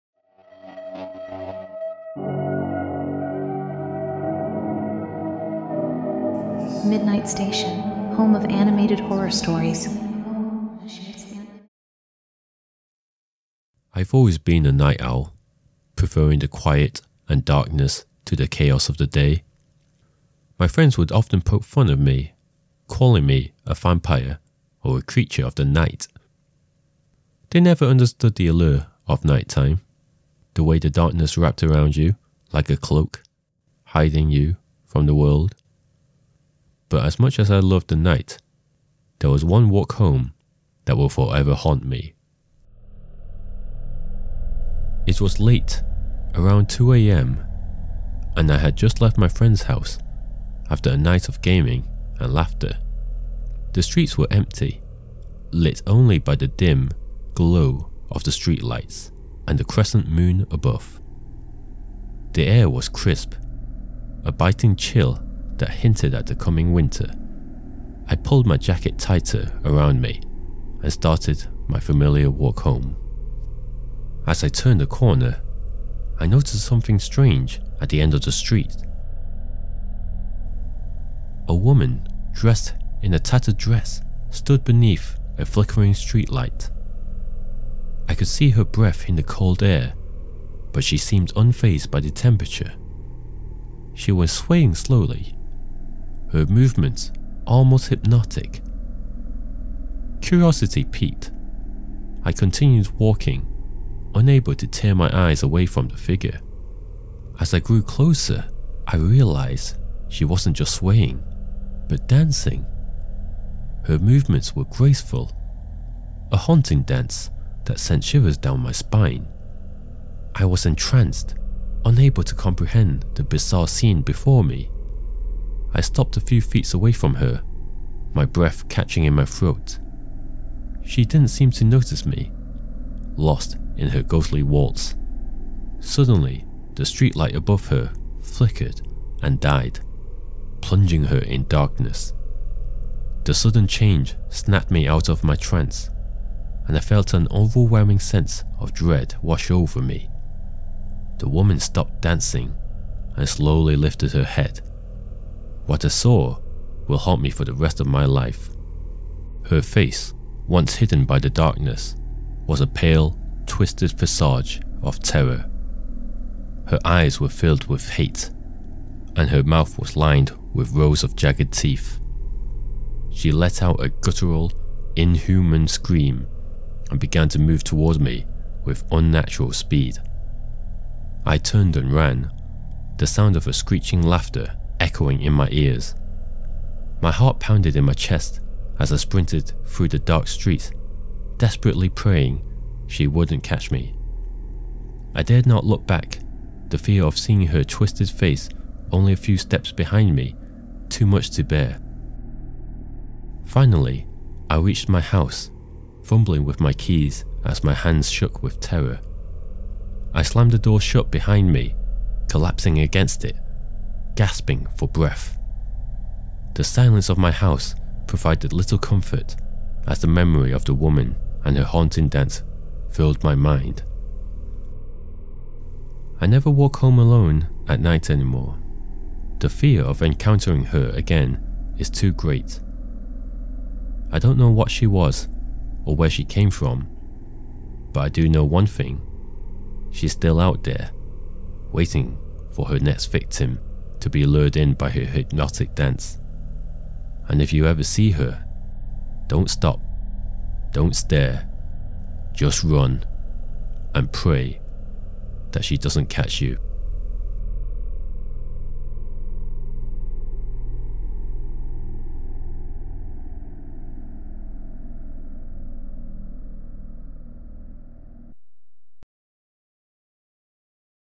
The Siberian Dancing Lady Project - An Animated Horror Story
In the dead of night, I walk home alone and encounter something unnatural - the Serbian Dancing Lady! Part of the Midnight Station [Animated Horror Stories]